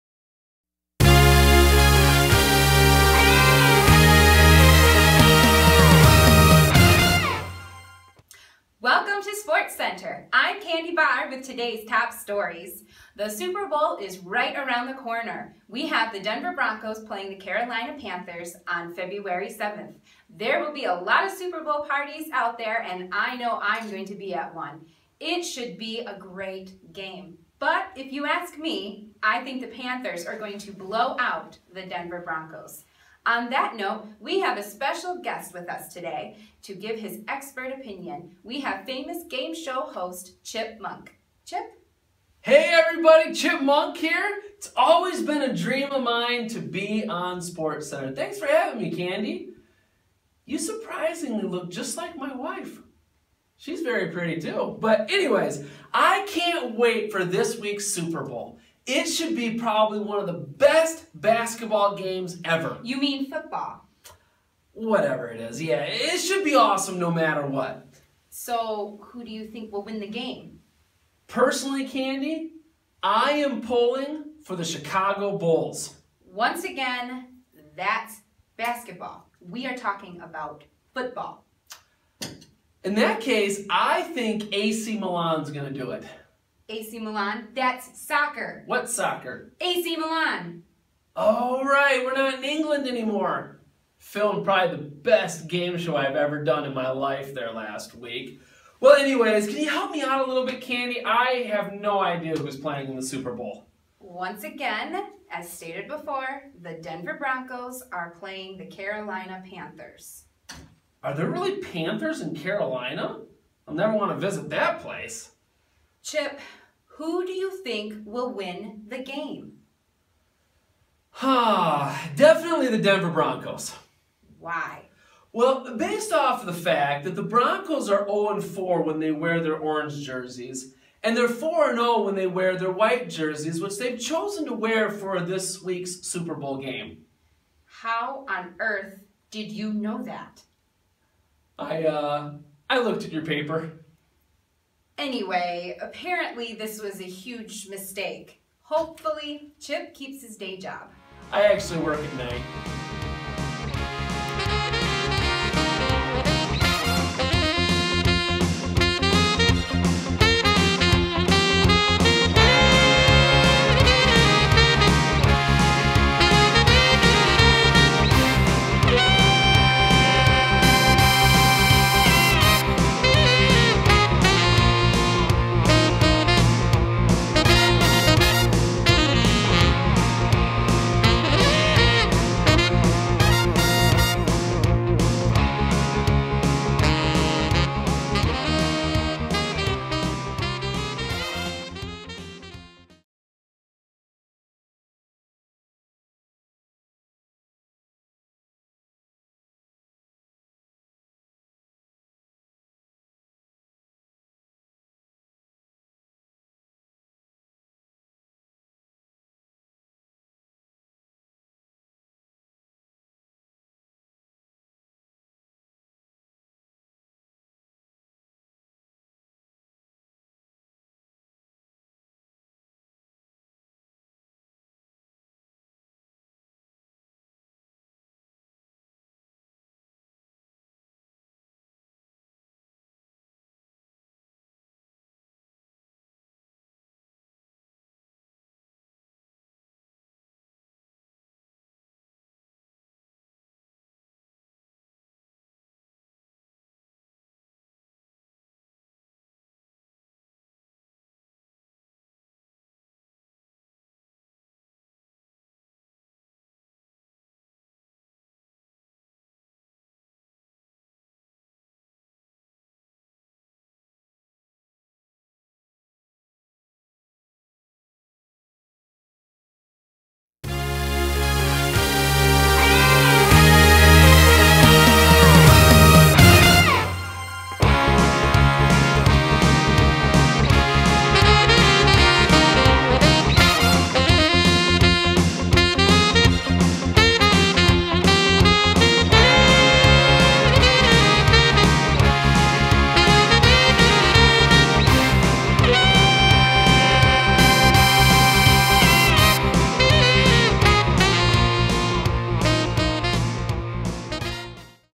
Superbowl 50
Come watch the Super Bowl with us at Calvary Bible Marshfield